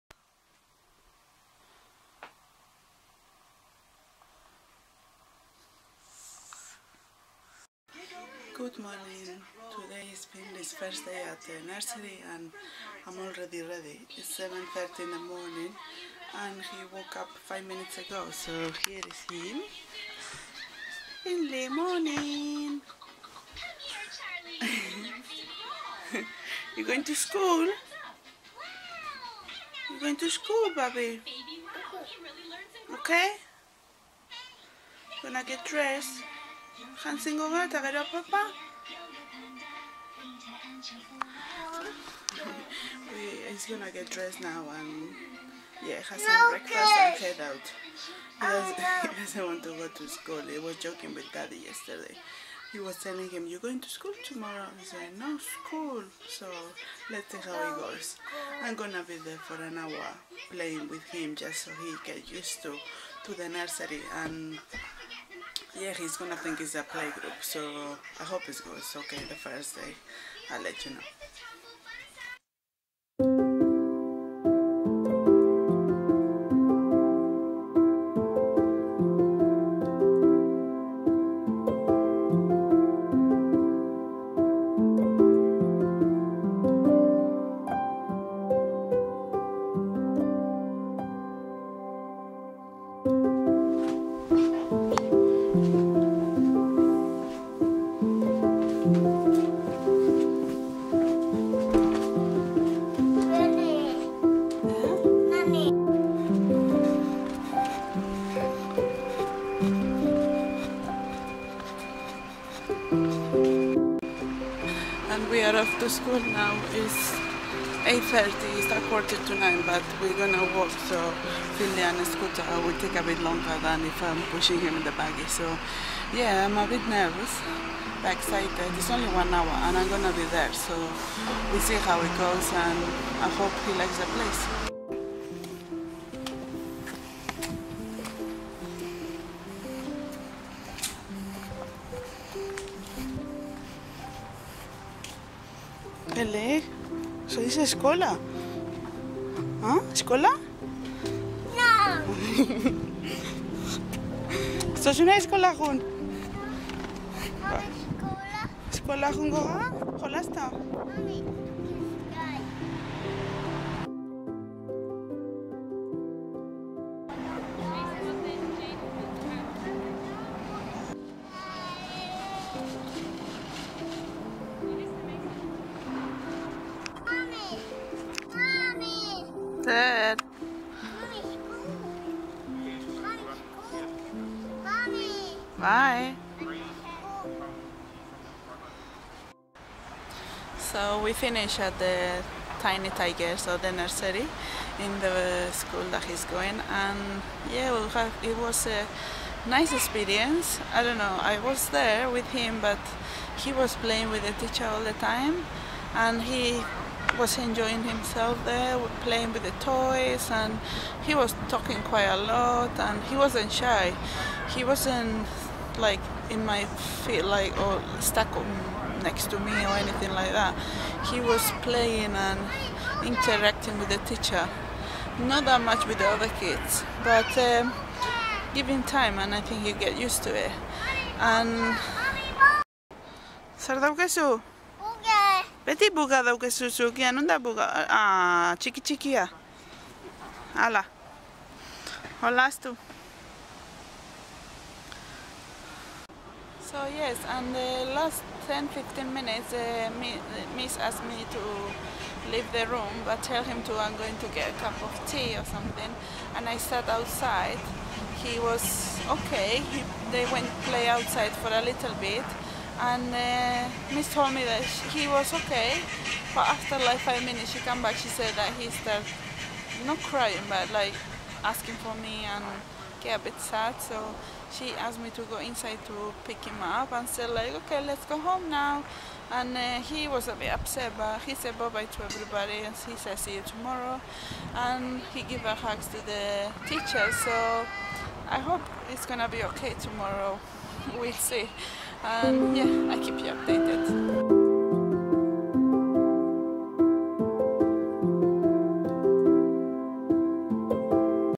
finley's first day at nursery/ Primer dia de guarderia de Finley sep17
This is Finley first day at nursery. I enjoyed the experience and he is getting use to it so lets see how is next week!!